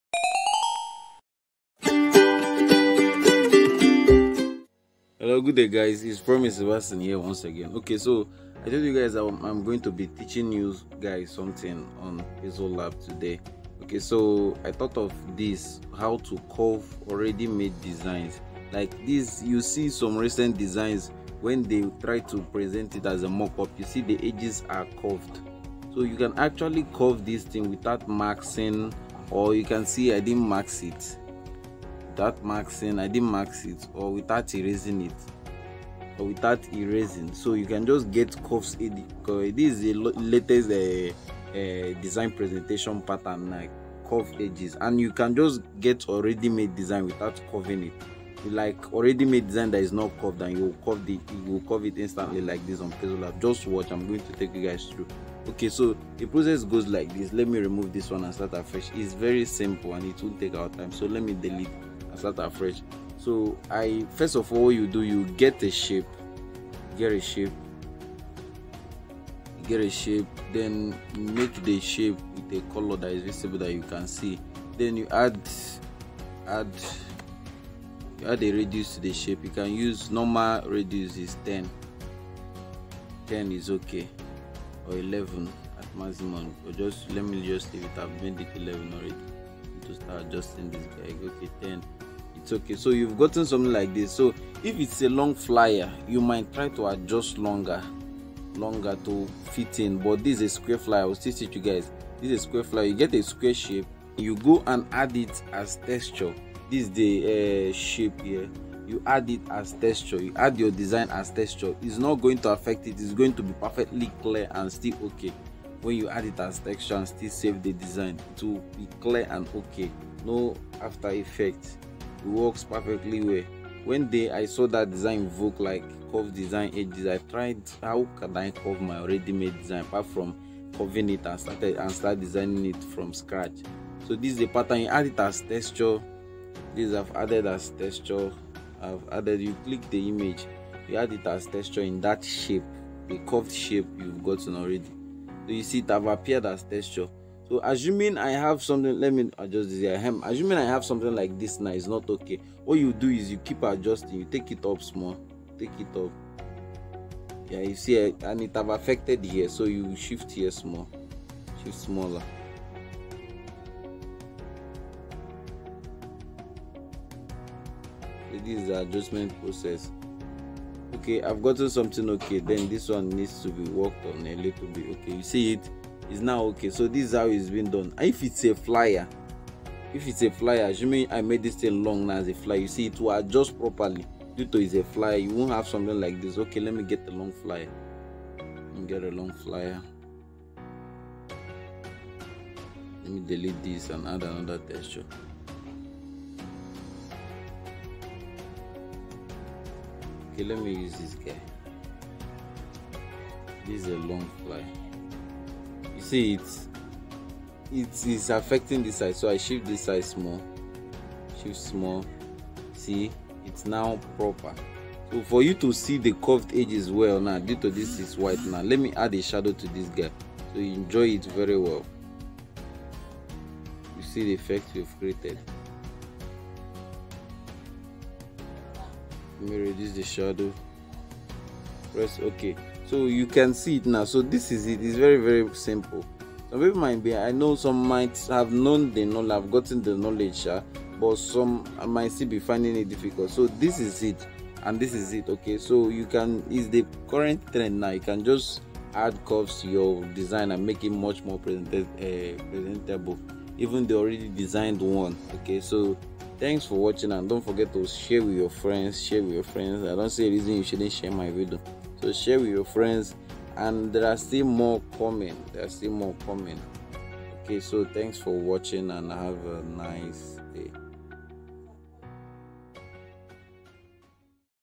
Curved mock-up Design presentation on pixellab
learn how to present your designs on pixellab with curved edges, it's easy and simple.